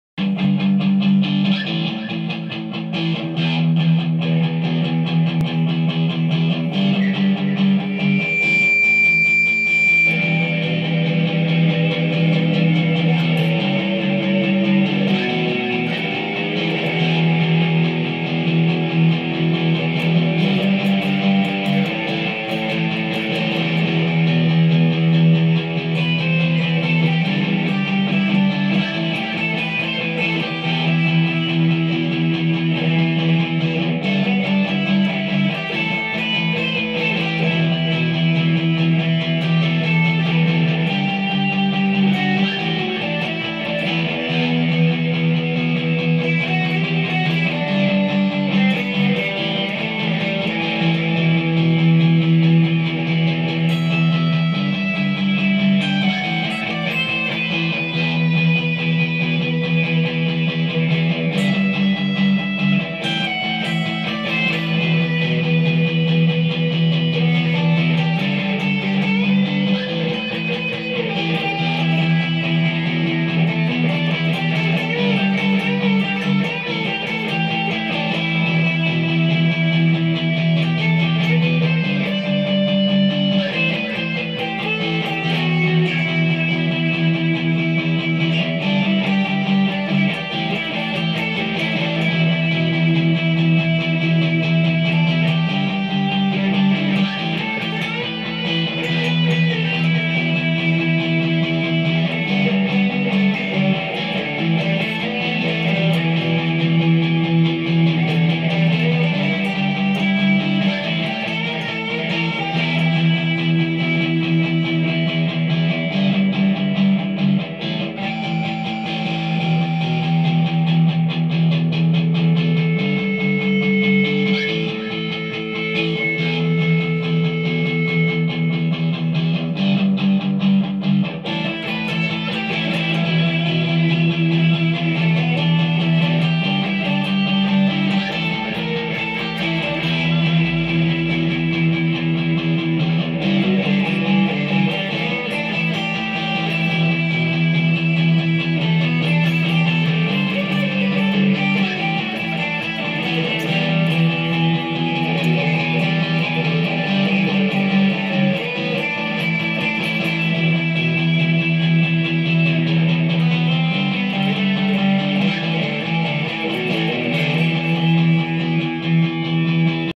First Act VW and Digitech RP355
VW played without boost through Digitech RP355 (looped rhythem part,) and Line 6 Spider Jam Amp .